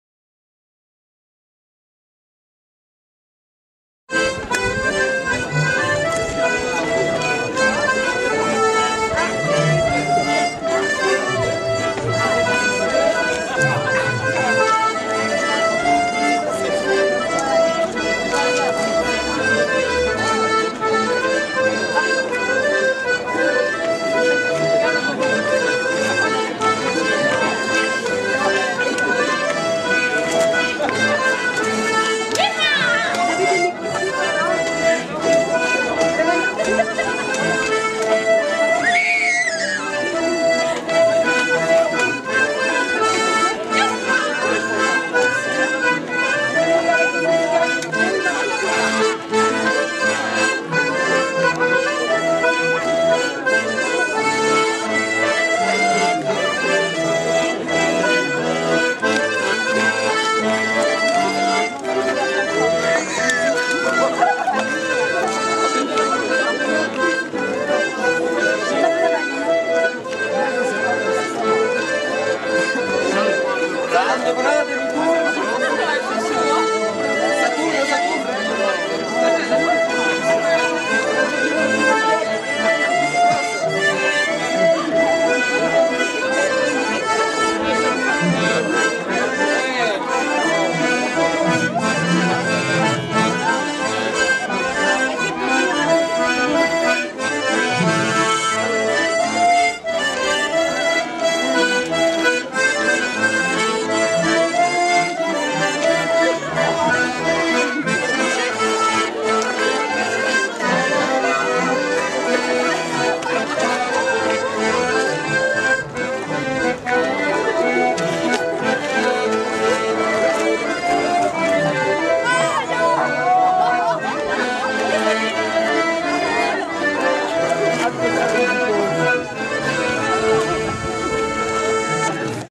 Accordéon Diatonique Scottish du Pays de Retz
Le "Cou'Acc" Sylvie, Patricia, Joël, Franck et Jean-Jacques le 23 Mai à la première édition de la fête des Utopiennes 31540 Mourvilles-Hautes.